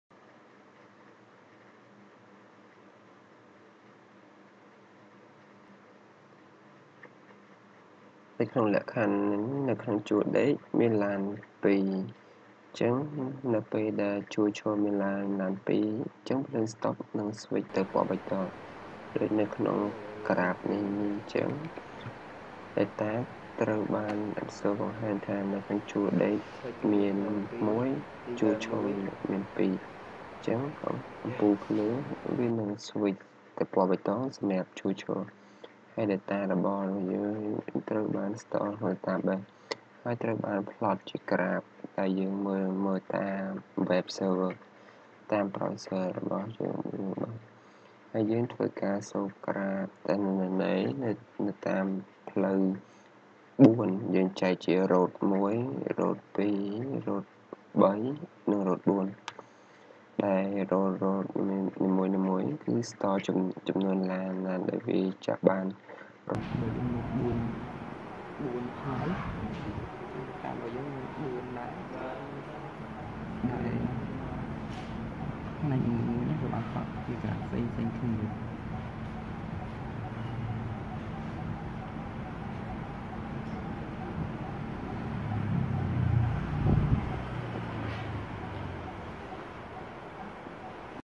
Live traffic light control Project Demo
Live Traffic Light Control project is my proposal project for Graduate of University.

The project has ability to control the traffic light. It has special algorithm to calculate and detect which have little or few vehicles will turn to Red light and the other road that hav many vehicles waiting will turn to Green light to reduce the traffic jam.